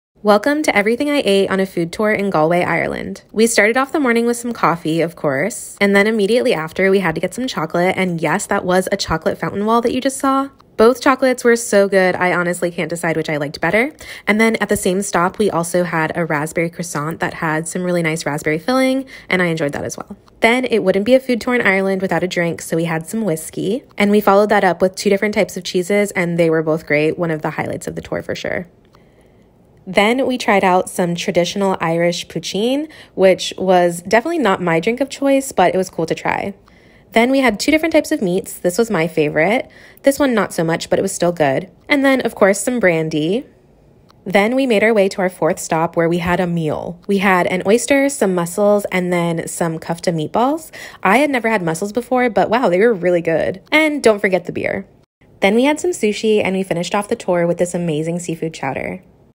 Everything I Ate In Galway Ireland
That seafood chowder was incredible! 🦀🐟🦞So grateful for BrendanVacations for organizing a trip with GalwayFoodTours